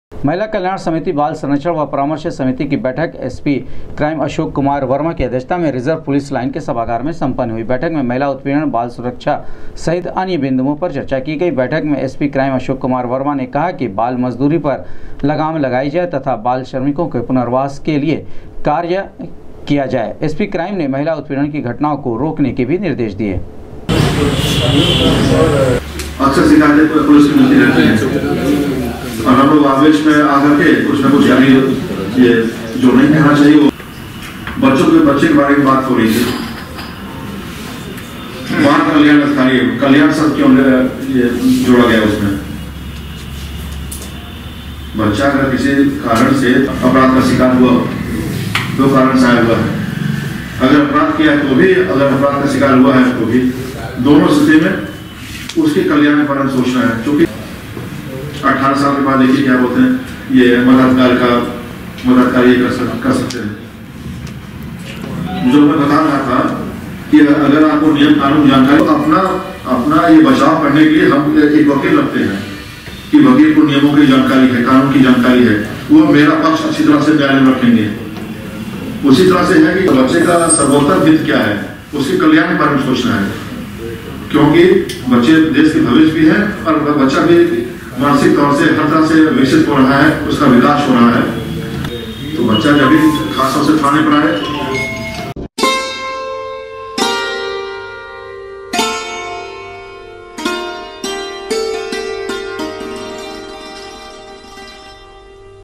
एसपी क्राइम की अध्यक्षता में सम्पन्न हुई महिला कल्याण,बाल संरक्षण व परामर्श समिति की
एसपी क्राइम की अध्यक्षता में सम्पन्न हुई महिला कल्याण,बाल संरक्षण व परामर्श समिति की बैठक, बाल श्रम रोके जाने सहित महिला उत्पीड़न की घटनाओं पर अंकुश लगाने के दिये निर्देश